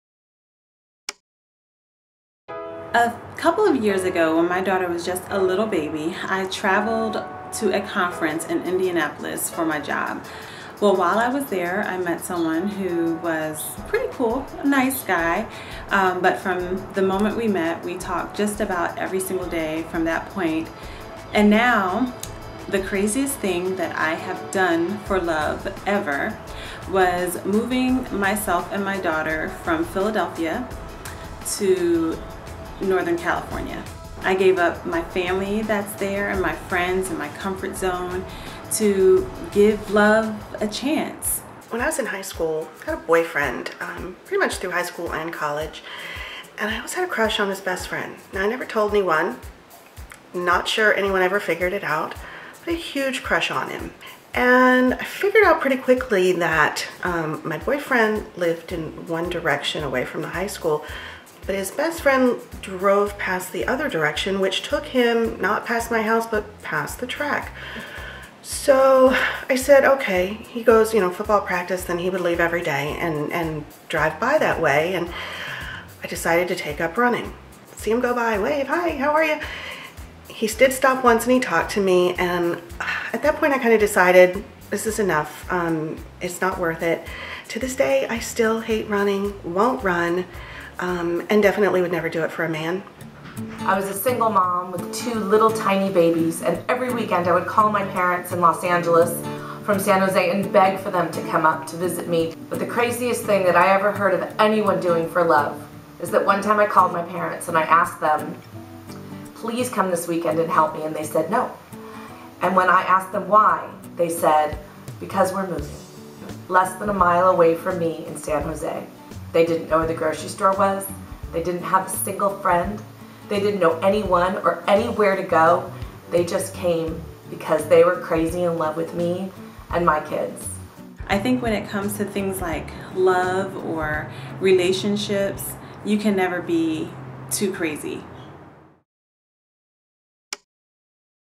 The Craziest Thing I've Done for Love Is...
They say love can make us do some crazy things! iVoices Brandi Jeter, Samantha Fein and Linda Sellers reveal the wildest thing they -- or someone else -- have done for love